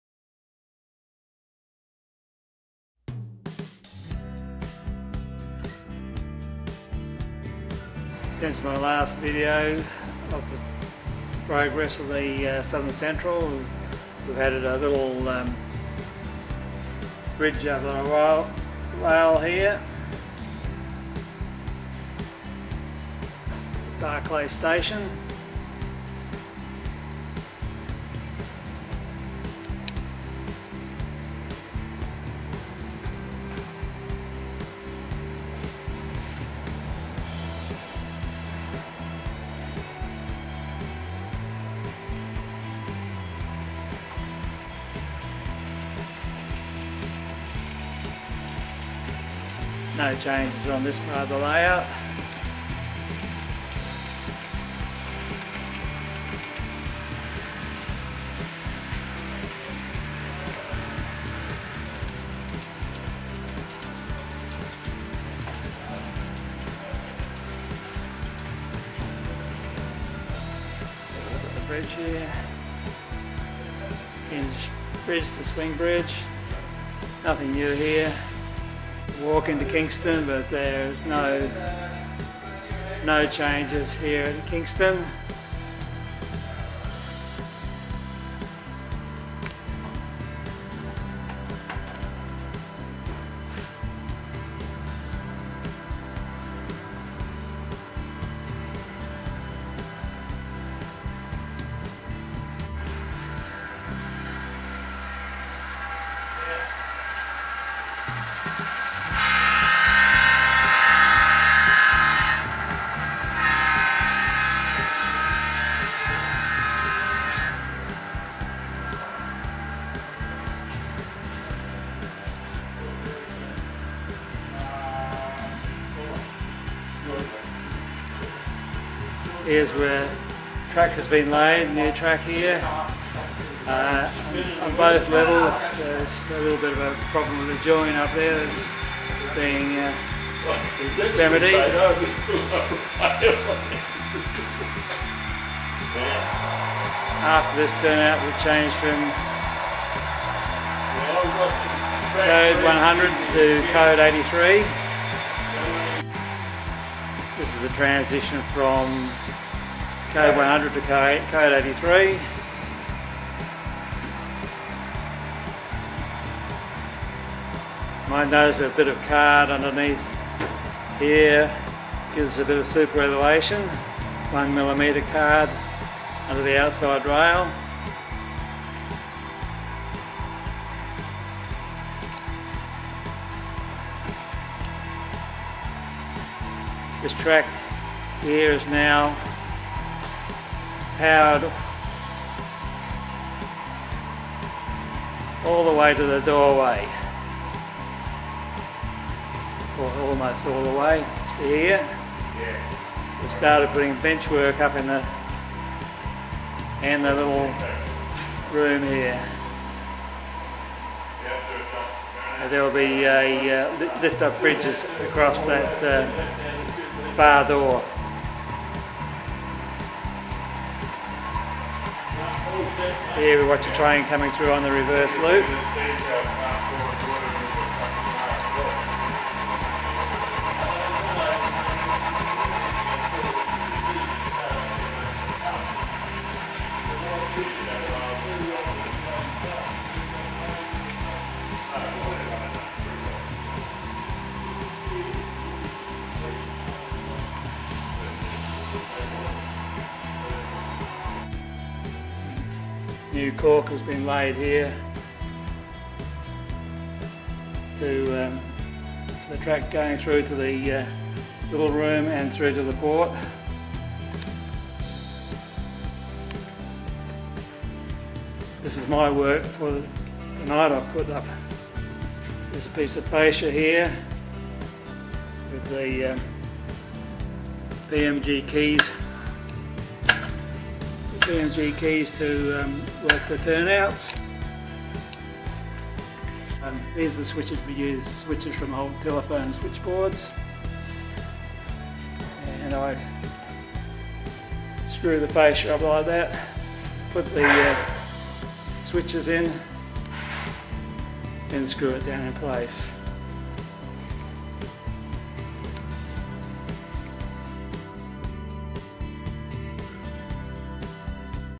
Under construction the SCRR 6
An update of progress on the Adelaide Model Railroaders HO scale layout. Plus a couple of run bys. We are using L-girder benchwork.